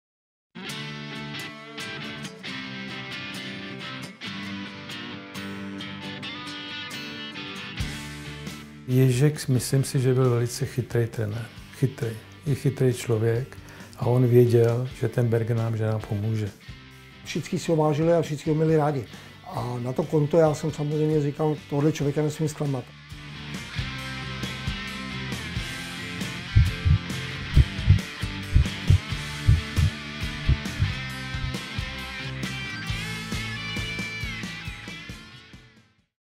Osobnosti páté éry - Berger, Hašek, Chovanec
Mezi výrazné osobnosti páté éry patří také Jan Berger, Jozef Chovanec a Ivan Hašek! Celé video